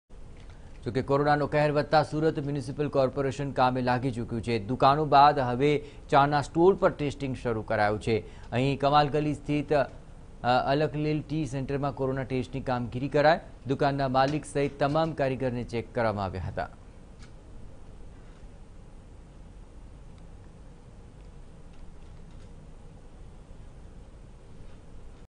Surat: Coronavirus testing drive conducted at various tea centres in Surat| TV9News
Surat: Coronavirus testing drive conducted at various tea centres in Surat|N12